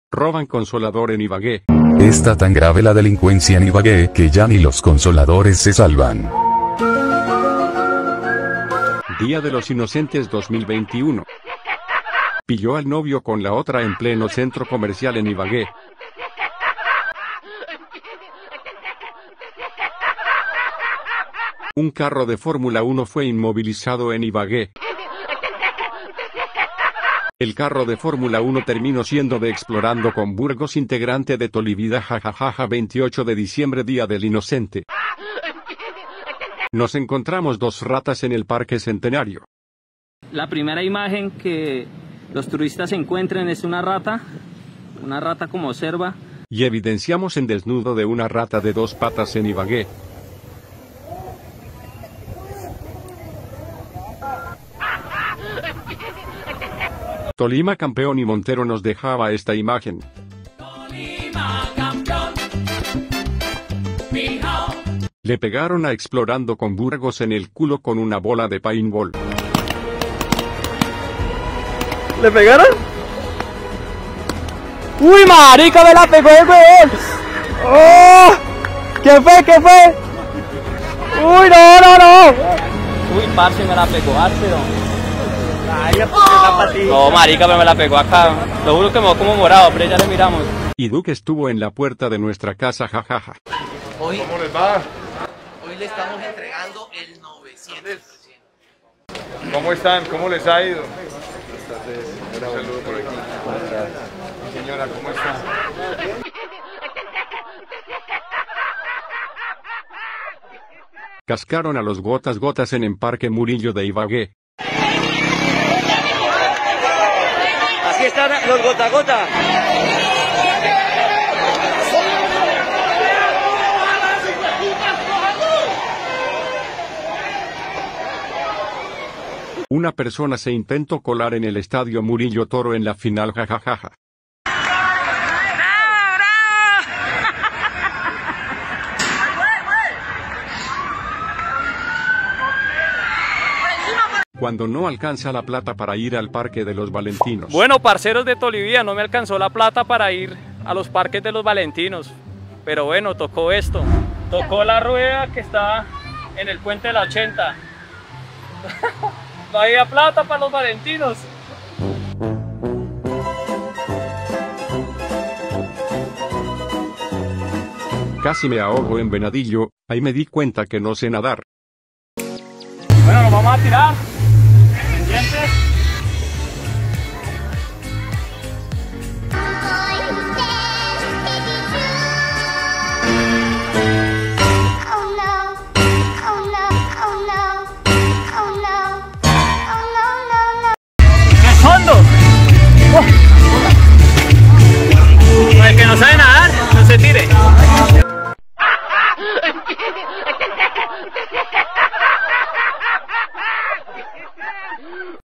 Ibagué-Colombia | Día de los inocentes 2021(28 DE DICIEMBRE)
Casos que nos paso en el Tolima y en algunas de Nuestras Grabaciones